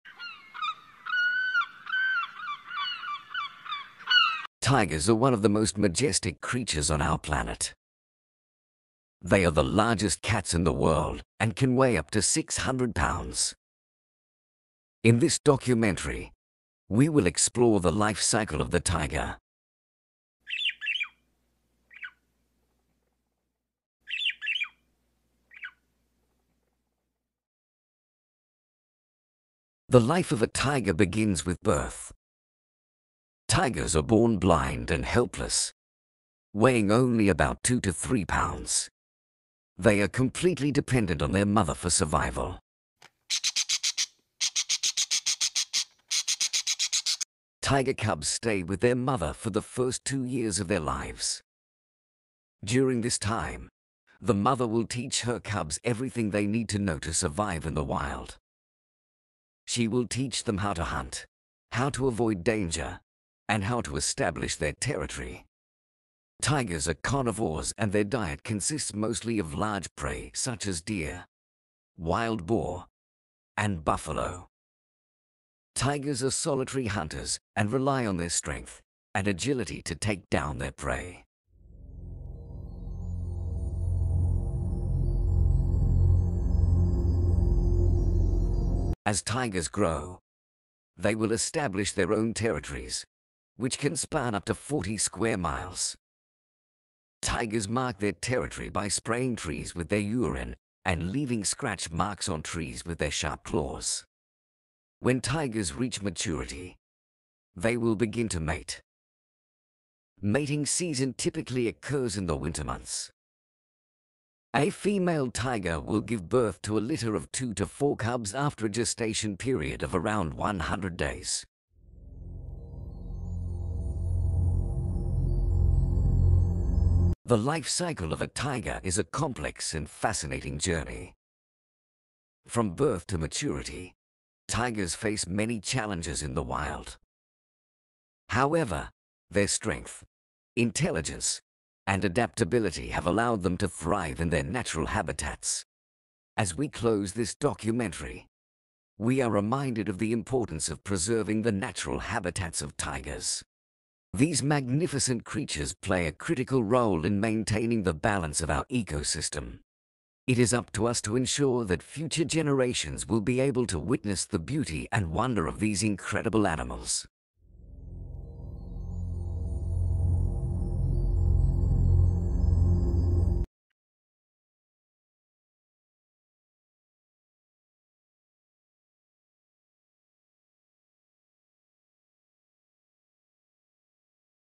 The Life Cycle of A Tiger
In this educational video, we explore the life cycle of one of the world's most majestic creatures: the tiger. From birth to adulthood, we will learn about the various stages of a tiger's life and the challenges they face along the way.

We start with the birth of tiger cubs and their early years, when they rely on their mother for protection, nourishment, and learning essential survival skills. As they grow, they become more independent and begin to explore their surroundings.

Next, we delve into the adolescent years, when young tigers must learn to hunt and defend themselves. This stage can be particularly challenging, as they must navigate their changing bodies and find their place in the social hierarchy of their pack.

As tigers reach adulthood, they become powerful predators at the top of the food chain. However, they still face threats such as habitat loss and poaching, which can endanger their existence.

Throughout the video, we will also highlight interesting facts about tigers, such as their distinctive stripes and how they communicate with one another. By the end of the video, viewers will have a better understanding of the fascinating life cycle of tigers and the importance of protecting these magnificent animals. So, join us on this journey through the life of a tiger and discover the wonders of this incredible species.